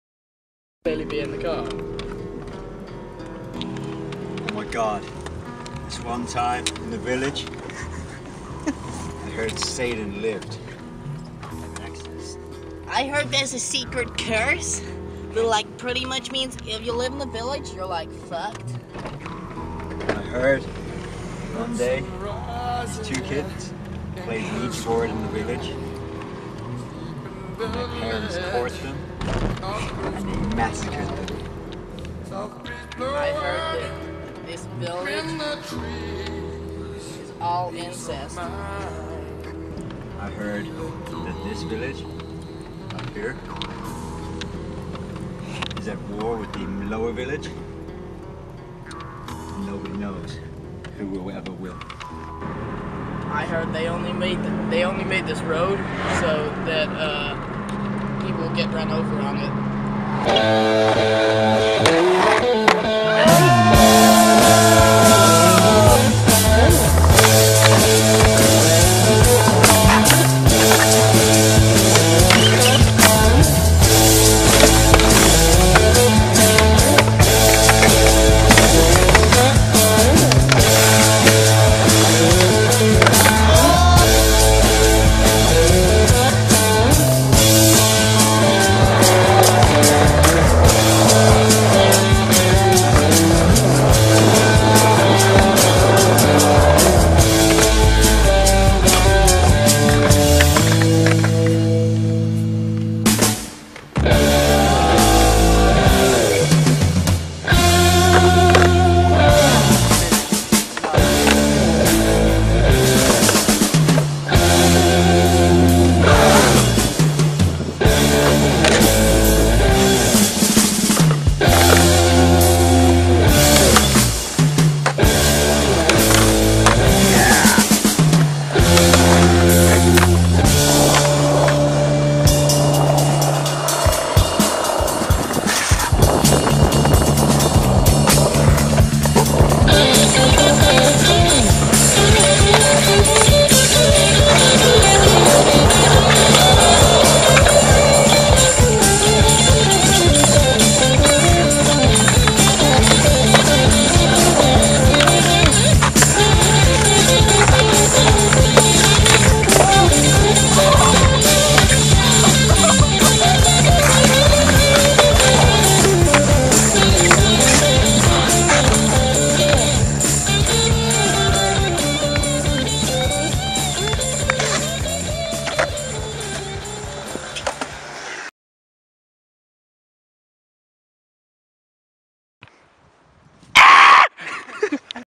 The Village
Some of the hommies have a shred at 'the village'.

Filming from:
Finn
Albie
Sam